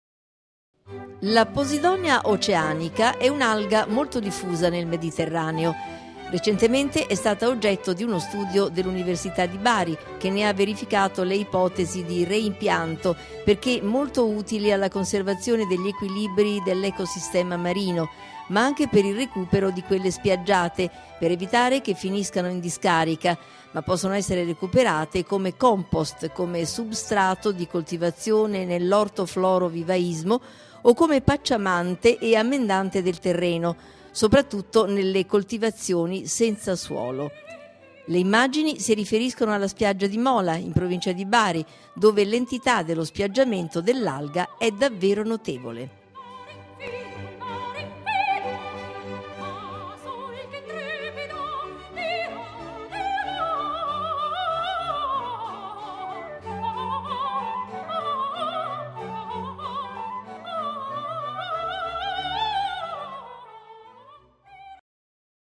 Un Alga per l'Ambiente marino e l'Agricoltura
Si chiama Poseidonia Oceanica e, secondo uno studio condotto da ricercatori dell'Università di Bari, è preziosa per l'ecosistema marino. Inoltre, quelle spiaggiate, considerate rifiuto maleodorante, da destinare alla discarica, potrebbero invece essere itilissime come concime naturale.